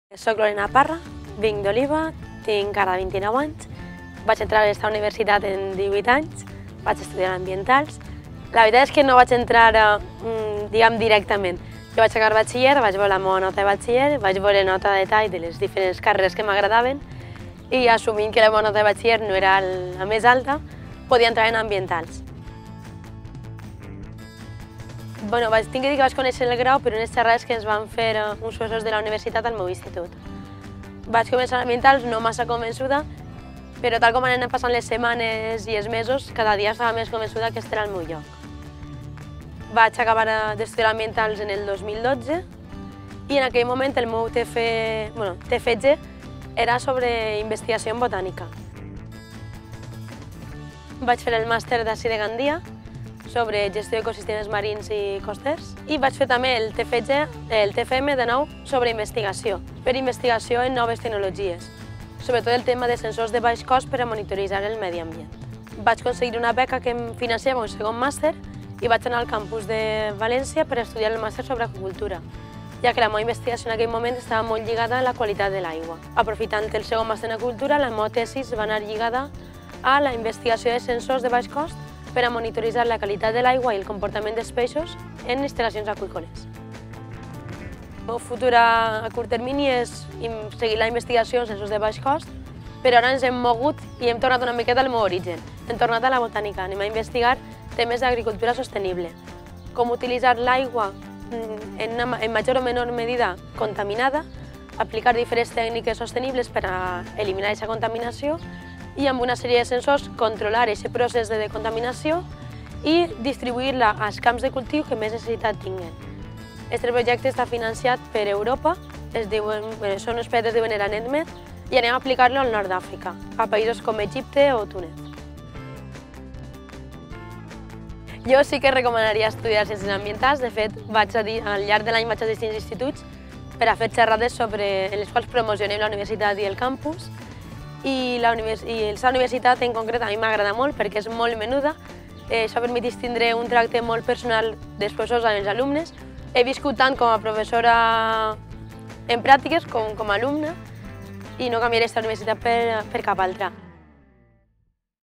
Lorena Parra, investigadora en l'àmbit de les Ciències Ambientals
Et presentem a Lorena Parra, natural d'Oliva, titulada en el Grau en Ciències Ambientals i Màster en Avaluació i Seguiment Ambiental d'Ecosistemes Marins i Costaners, tots dos en el Campus de Gandia; també és Màster en Aqüicultura, en el Campus de Vera UPV.

Tesi doctoral
Contribuir a una aqüicultura més sostenible: aquest és l'objectiu final de la recerca desenvolupada per l'olivera Lorena Parra i dirigida pels investigadors de la Universitat Politècnica de València en el Campus de Gandia, Jaime Lloret i Miguel Rodilla. Amb aquesta finalitat, el treball presenta el disseny i desenvolupament de sensors per a piscifactories, de baix cost, amb un consum molt reduït d'energia, que controla fins a 10 paràmetres de la qualitat de l'aigua;  aquests dispositius monitoritzen el comportament dels peixos, especialment durant el període d'alimentació i permeten l'ajust automàtic de l'alimentació dels peixos.

En el treball, arreplegat en una tesi doctoral, es demostra l'aplicació d'aquests innovadors sistemes,  tant en piscifactories com en manglars i estuaris; el procés de monitoratge inclou, a més, un sistema intel·ligent per a controlar i rastrejar la contaminació en els cossos d'aigua.